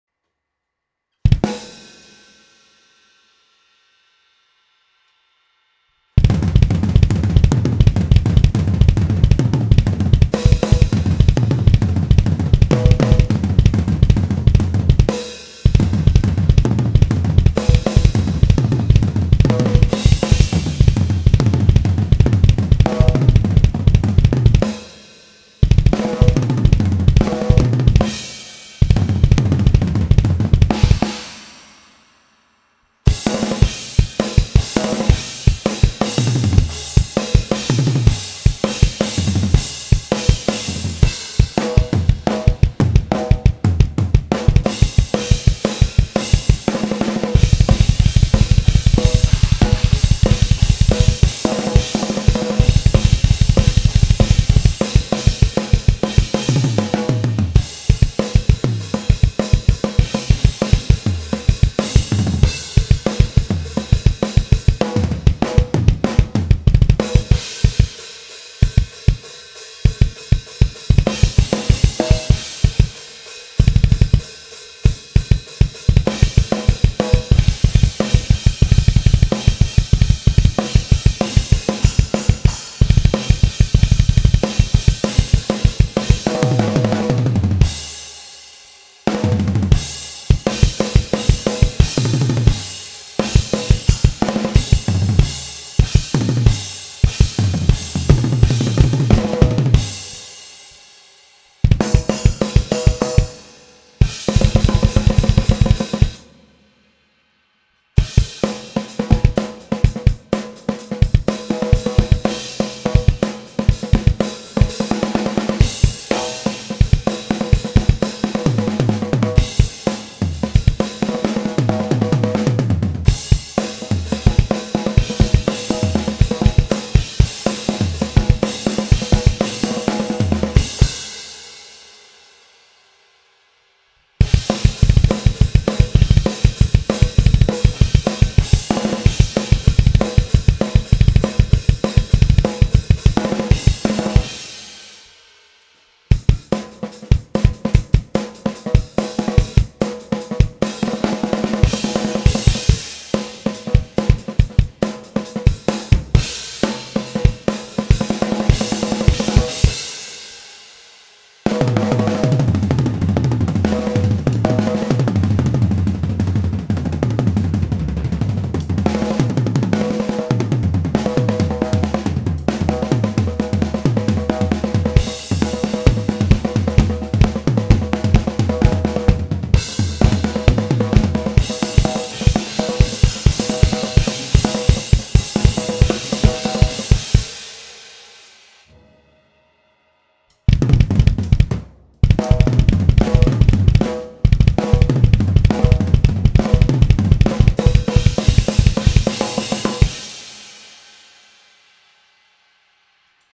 Mic checking
Seeing how some new mics sound.. really rough. Recorded, adjusted levels, put with video all in about 30mins